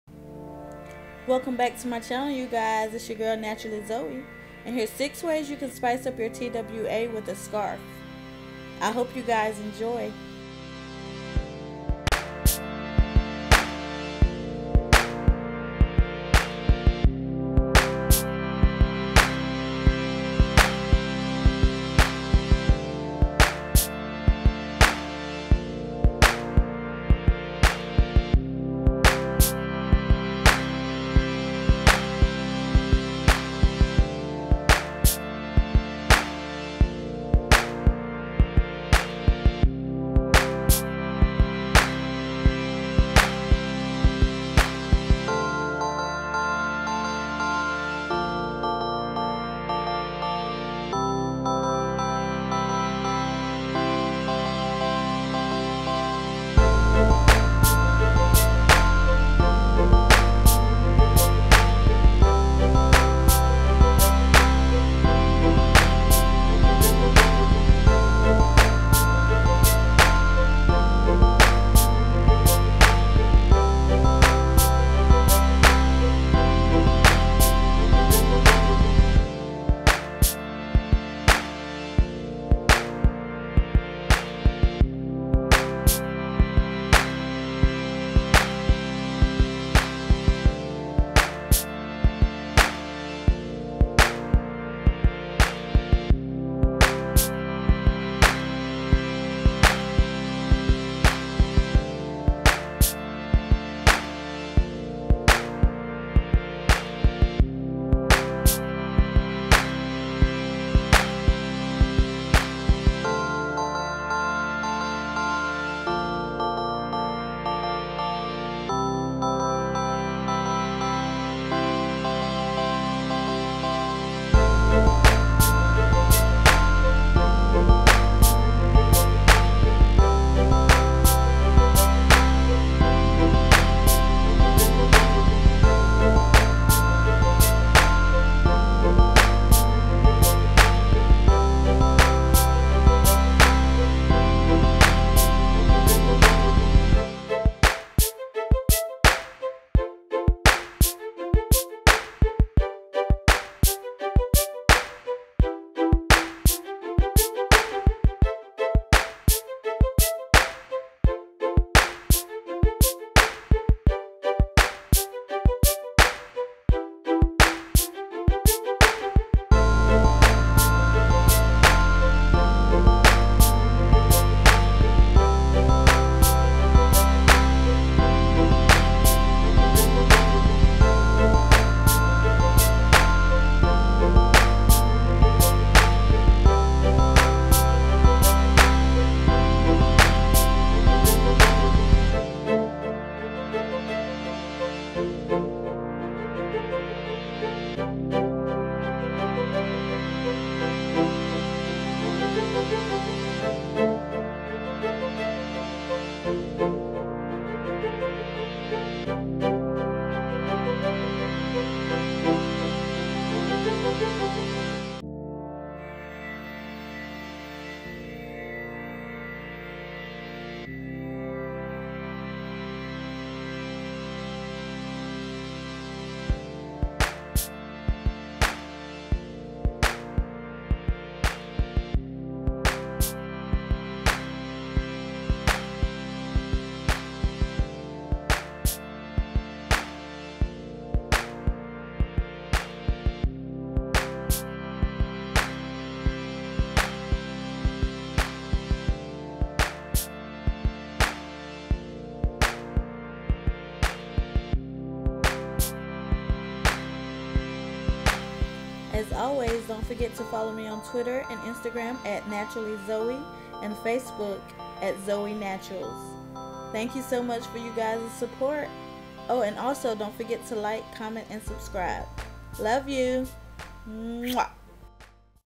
NATURAL HAIR | SPICE UP YOUR TWA -FINGERCOILS EDITION : 6 WAYS TO WEAR A SCARF
Hi Loves, it's another video for my beautiful people!!! :-D I hope you all are doing well. So this video is based off how I style my TWA. I finally tried fingercoils, and I was trying to figure out how to style my hair. So I came up with these 6 WAYS TO SPICE UP YOUR TWA USING A SCARF | FINGERCOILS EDITION. I really hope you enjoy!!!

Love you guys. MUAH :-*

Soundtrack Original by VapeBeats